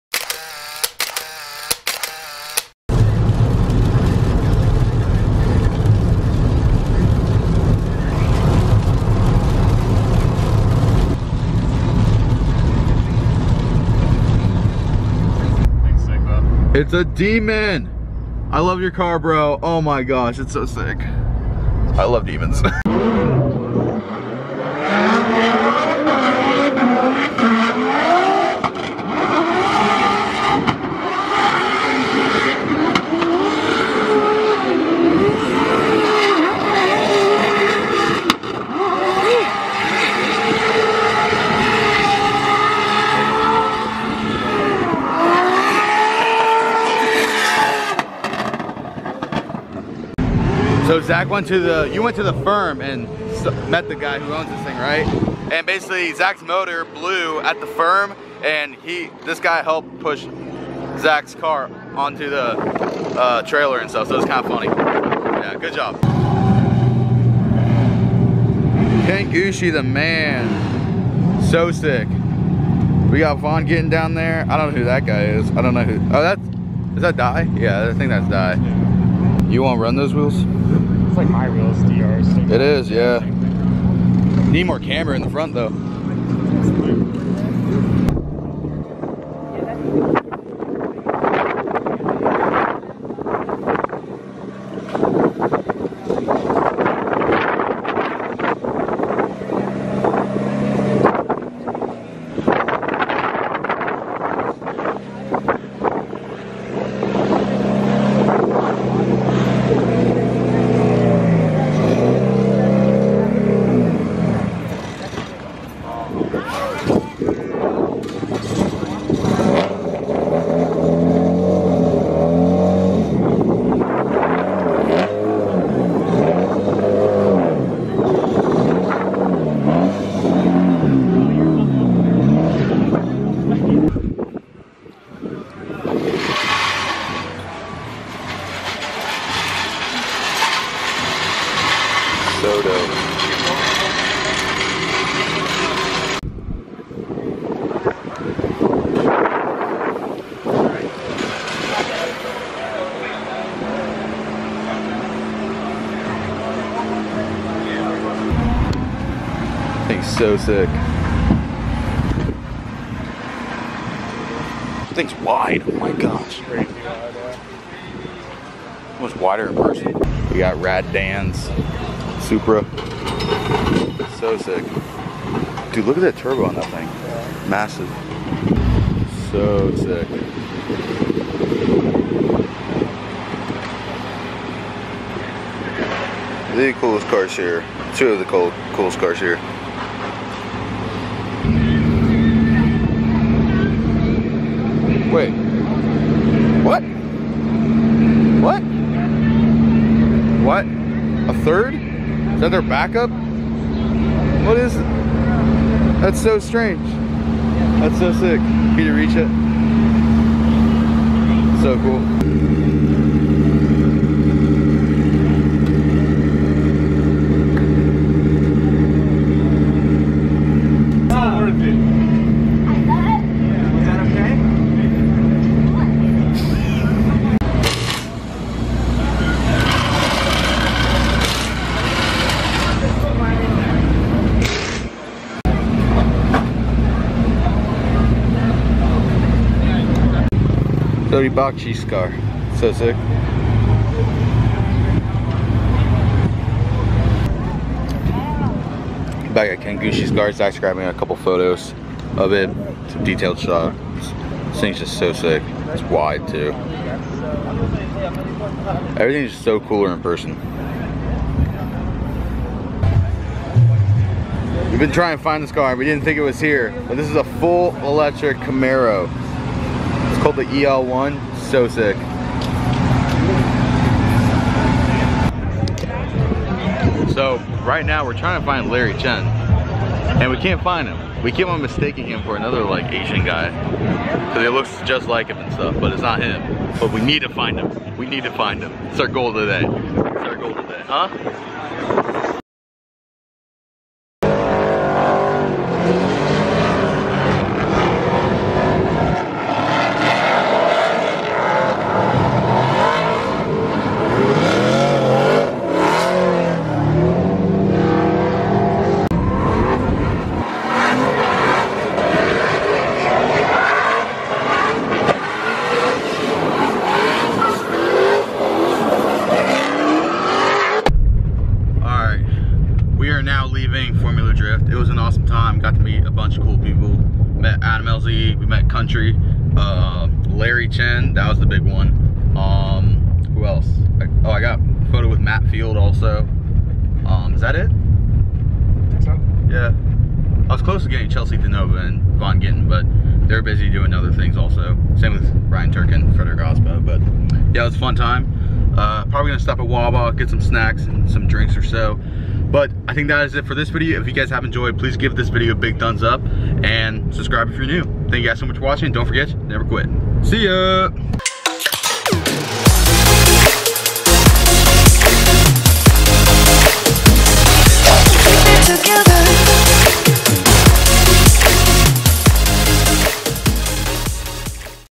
Formula DRIFT Orlando!!!!
Zach and I went to Formula Drift Orlando! We met some awesome people! Watched some people rip! and got some good photos too! Hope you enjoy!